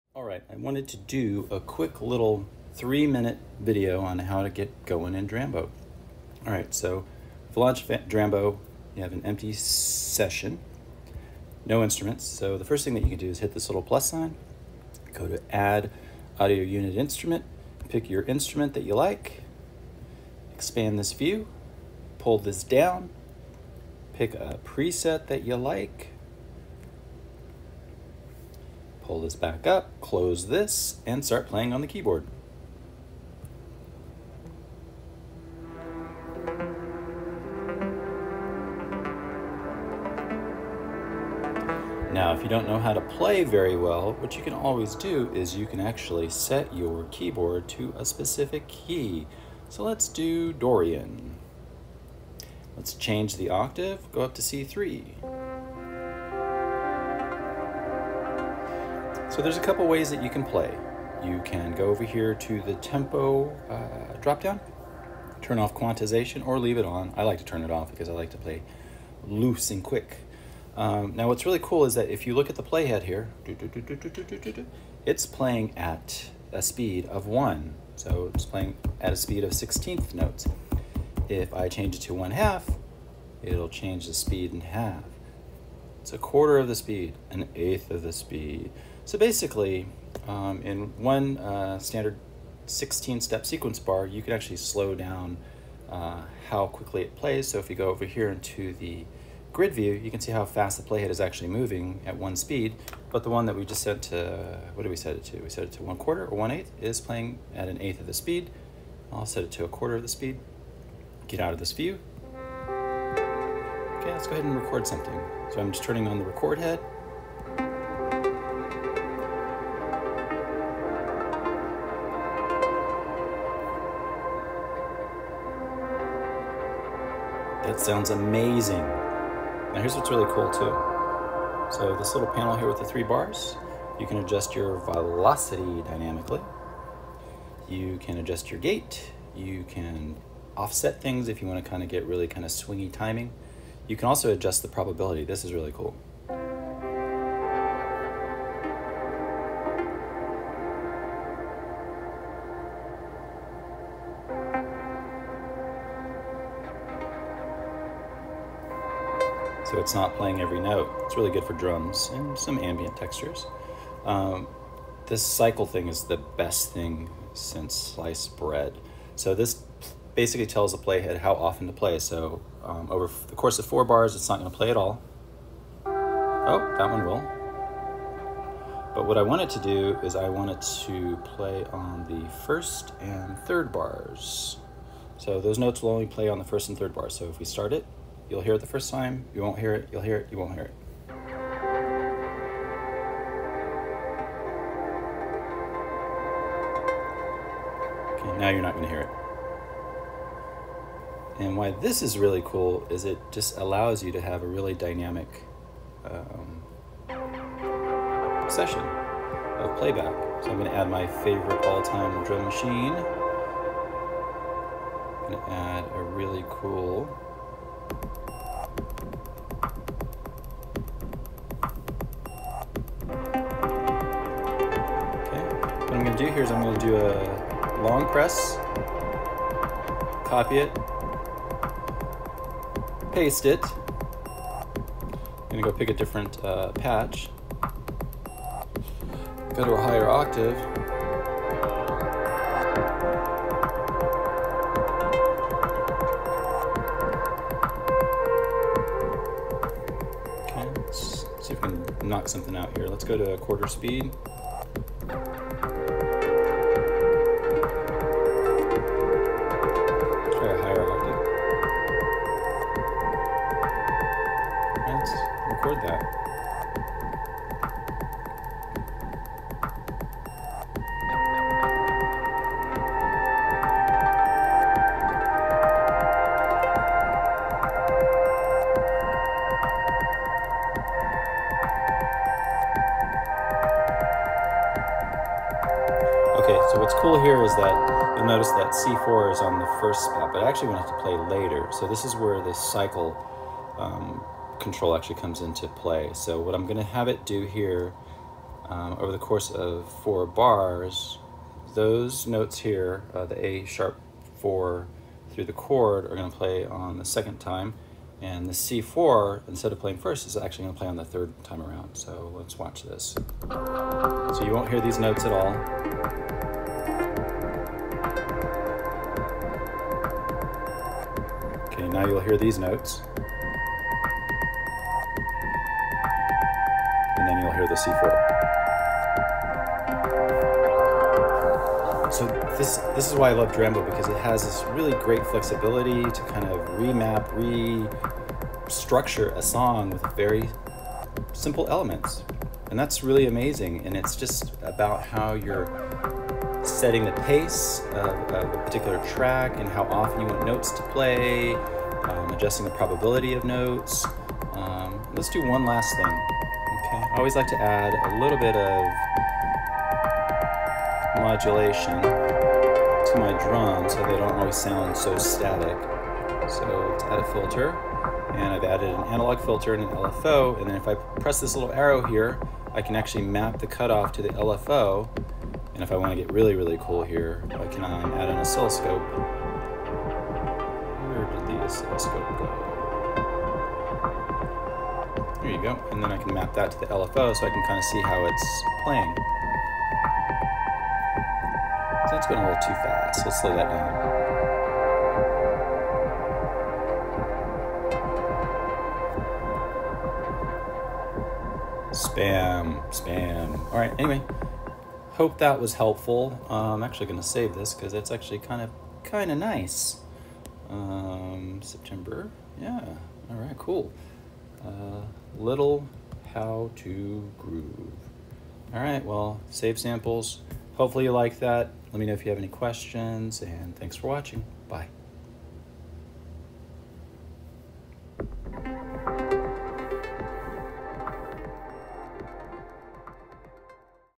drambo quick start: auv3 host + onboard effects
A video showing how to easily load an auv3 into Drambo